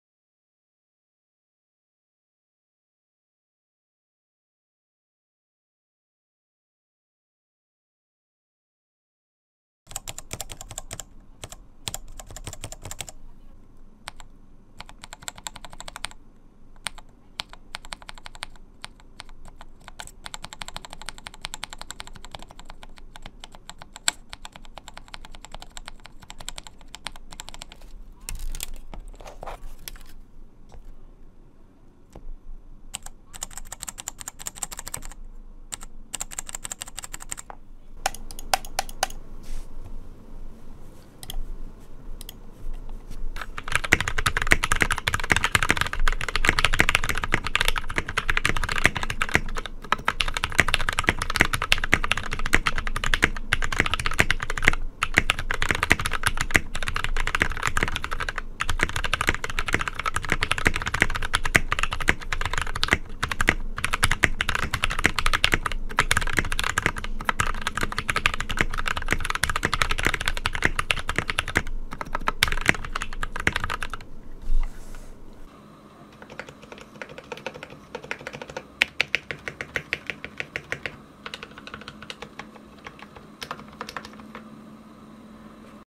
Custom Modified Royal Kludge (RK 68)
Case & PCB: RK68 Wired
Switches: Lubed Gatern Browns (Glorious lube)
Stabilizers: Lumia Stabilizers lubed (Glorious lube and direlectric grease)

Wired: USB Type-C

Keyboard came equipped with scratchy sounding off-brand gateron brown switches. They were soldered into the board. I have gone ahead and desoldered them and added in lubed gateron brown switches.